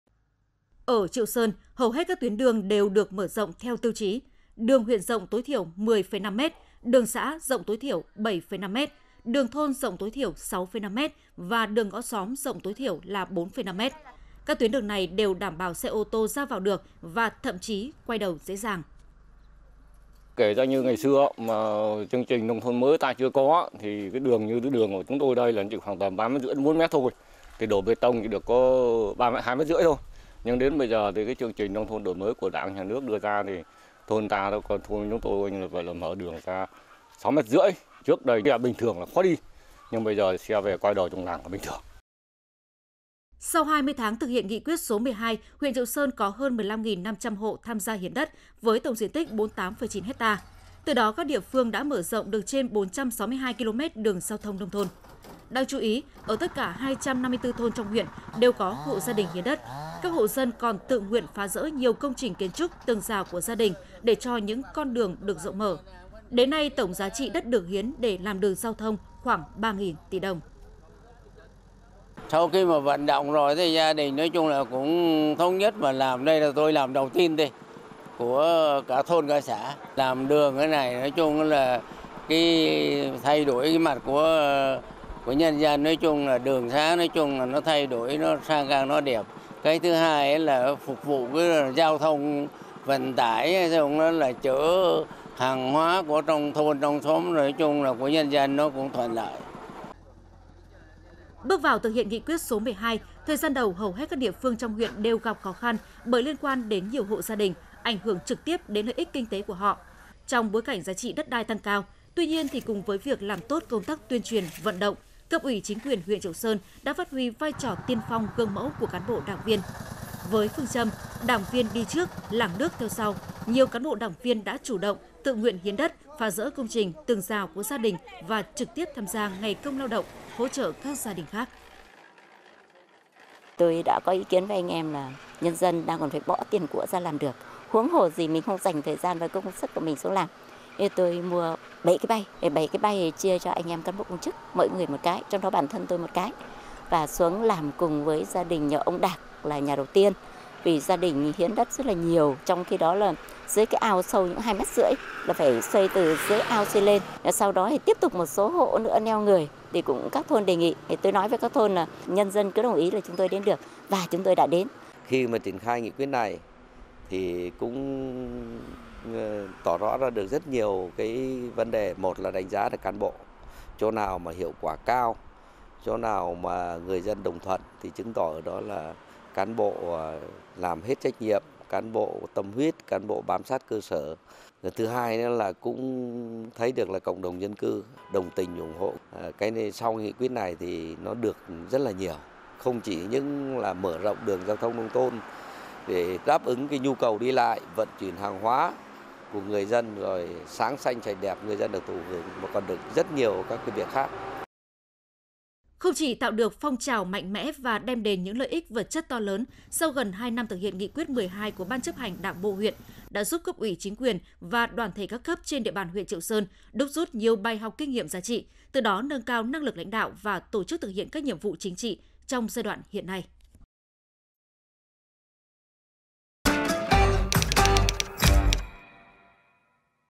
HIẾN ĐẤT LÀM ĐƯỜNG GIAO THÔNG NÔNG THÔN: LÒNG DÂN RỘNG, LÒNG ĐƯỜNG THOÁNG | VTV5
Ở Triệu Sơn, hầu hết các tuyến đường đều được mở rộng theo tiêu chí: Đường huyện rộng tối thiểu 10,5m, đường xã rộng tối thiểu là 7,5m, đường thôn rộng tối thiểu 6,5m và đường ngõ xóm rộng tối thiểu là 4,5m. Các tuyến đường này đều đảm bảo xe ô tô ra vào được, và thậm chí quay đầu dễ dàng.